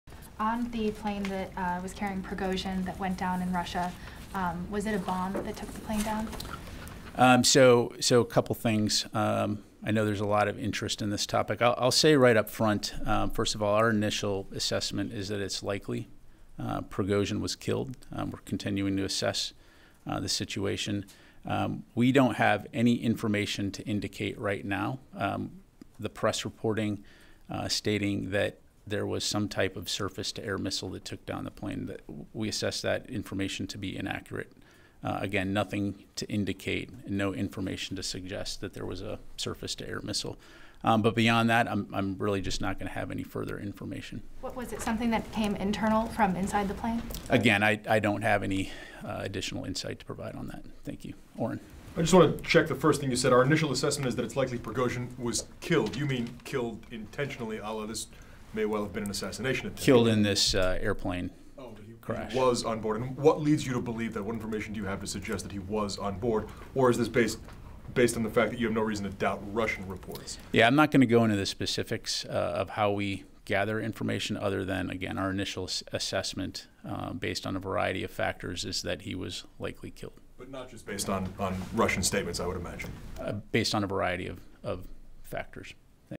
Pentagon on Death of Wagner Group Leader Yevgeny Prigozhin
Pentagon spokesman Brig. Gen. Patrick Ryder on Thursday said it was "likely" that former Wagner Group leader Yevgeny Prigozhin was killed when his plane crashed north of Moscow on Wednesday.

The spokesman said the Pentagon was continuing to assess the situation, but it had seen no evidence that a surface-to-air missile struck the jet of the mercenary leader who staged a short-lived rebellion against the Kremlin in late July.